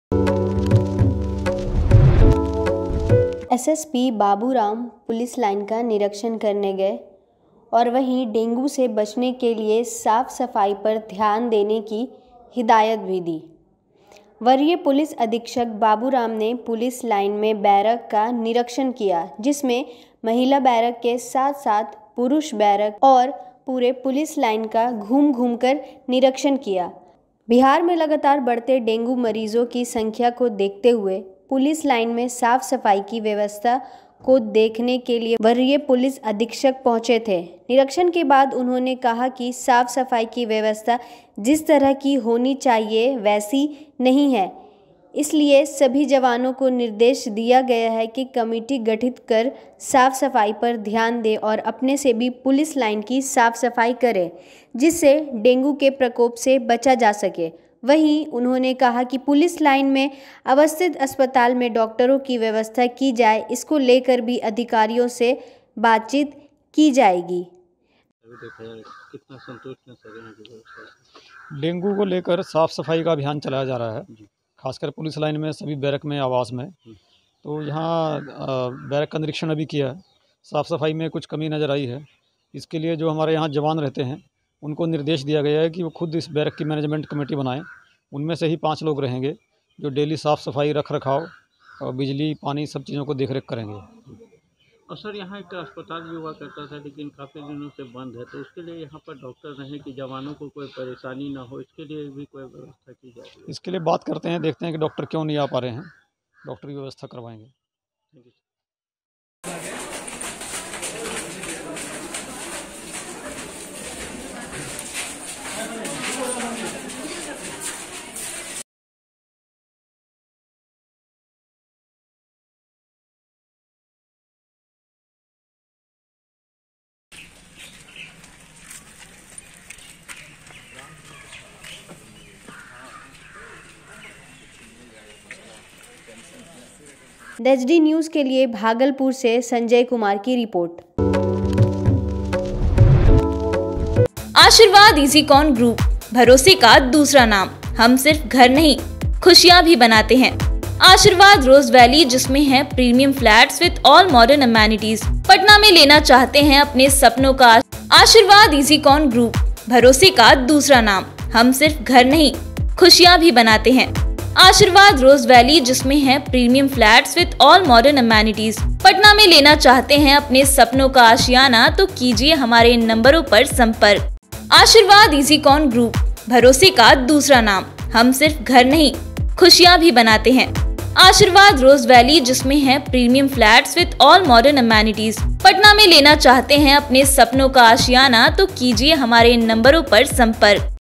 BhagalpurSSP BabuRamने किया औचक निरिक्षण,डेंगू से बचने के लिए साफ सफाई पर ध्यान देने का दिया निर्देश
Edited By: Akash kumar,Himanshu kumar
Anchor: